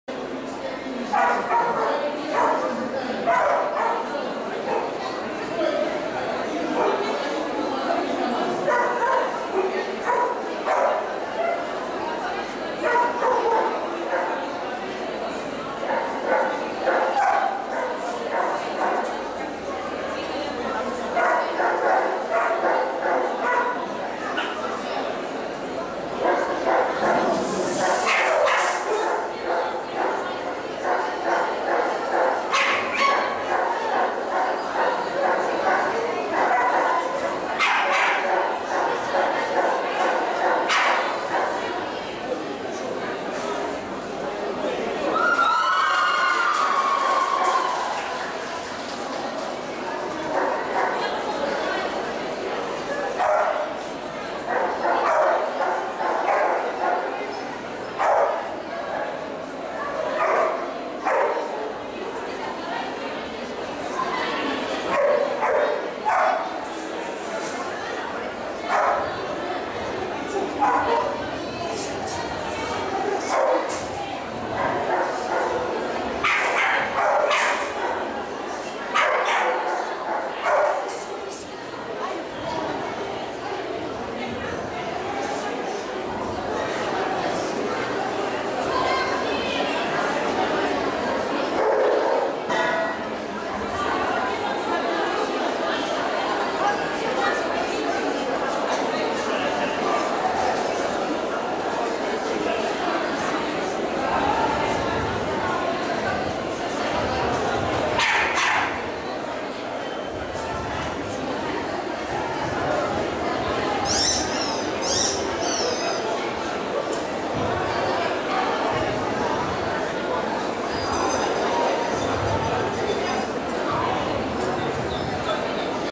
Dog Show «Весна Сибири» p03
стандартные
юниоры

Артур Из Замка Ланселота - JCAC
Хрустальная Душа Большая Медведица - JCAC, ЛЮ

judge: Korotaeva S.
Всероссийская выставка собак всех пород «Лидер Сибири - 2012»